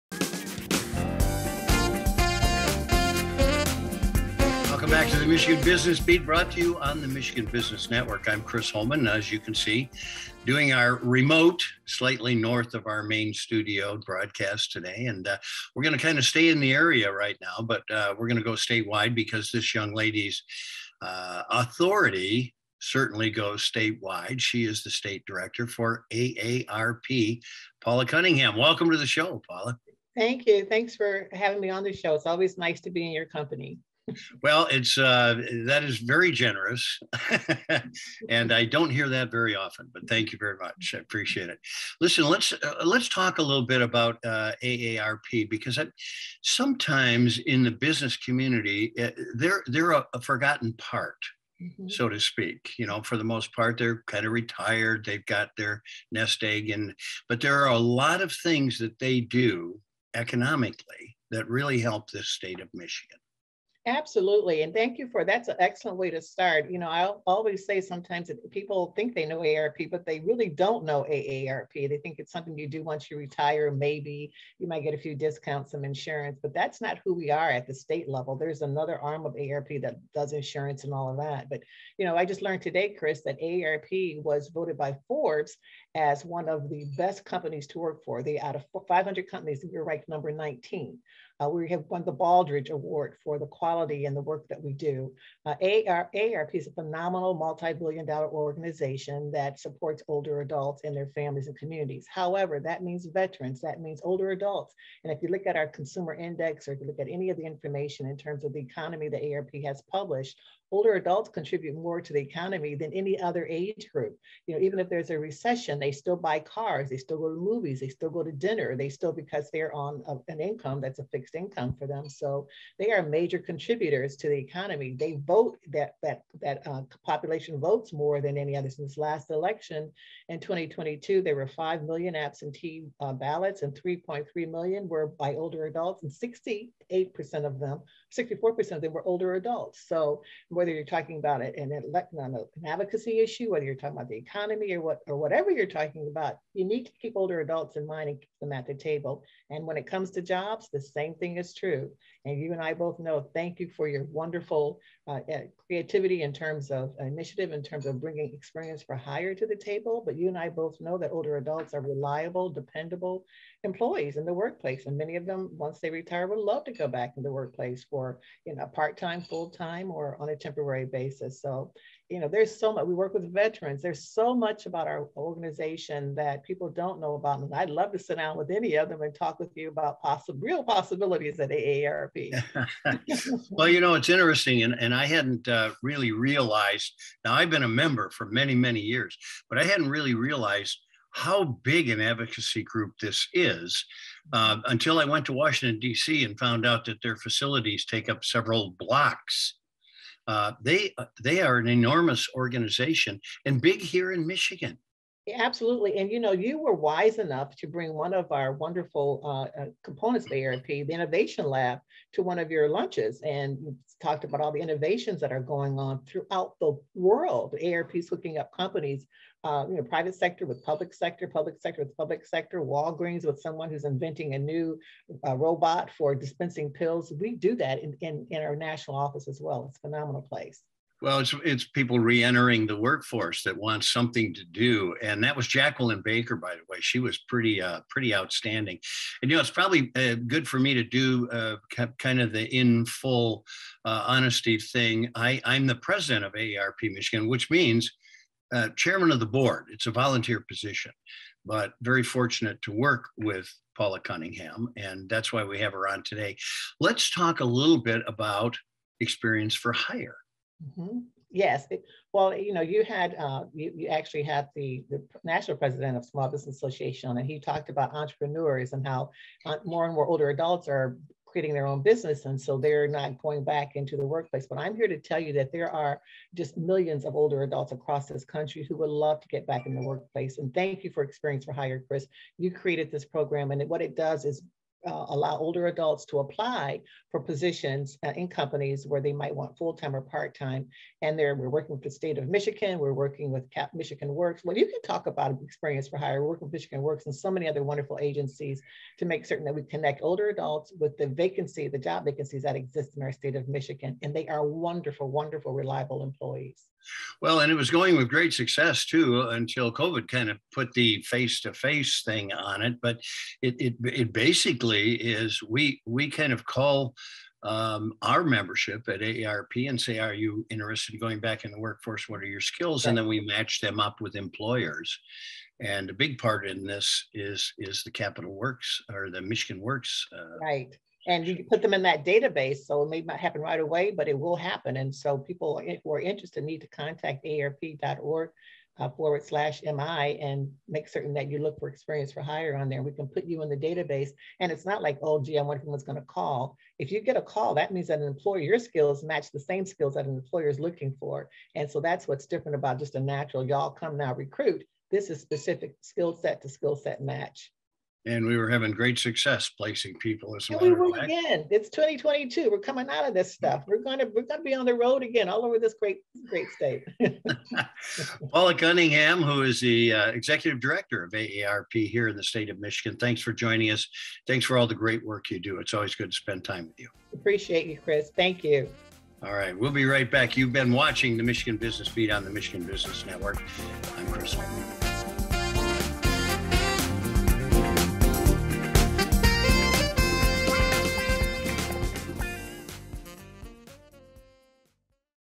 Michigan Business Beat | AARP Michigan, Updates, and Details on Experience for Hire.
Chris Holman catches up with Paula Cunningham, State Director, AARP Michigan, Lansing, MI.
Paula talks AARP Michigan's various services and then about their Experience for Hire program and why employers should not forget about older adults when it comes to hiring.

Experience for Hire
Experience for Hire is a collaborative project intended to rethink hiring in Michigan by connecting employers with experienced, qualified job seekers age 50 and over who are seeking full- or part-time opportunities to put their valuable skills to work.

Experience for Hire leverages the connections and capacities of partner organizations to help employers match with Michigan's growing mature workforce to find qualified job seekers for available positions. The program provides employers a convenient opportunity to interview applicants who bring valuable experience, require little job training, and are open to a flexible schedule.